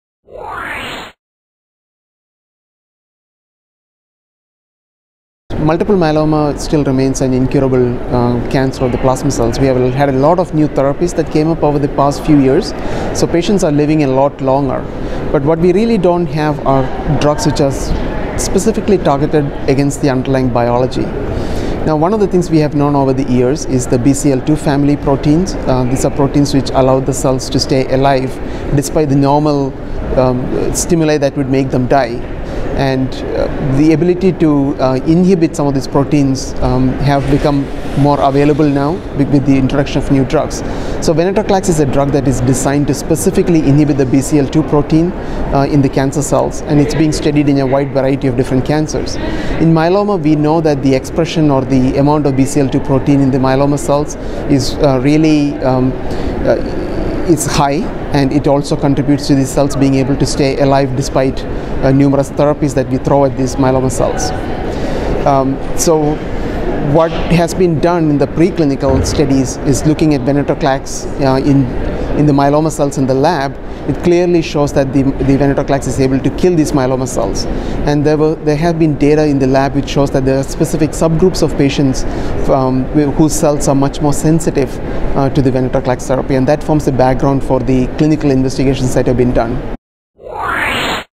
Dr. Kumar on the Rationale for Studying Venetoclax in Multiple Myeloma
Shaji Kumar, MD, professor of Medicine, Mayo Clinic, discusses the rationale for investigating venetoclax as a treatment for patients with multiple myeloma.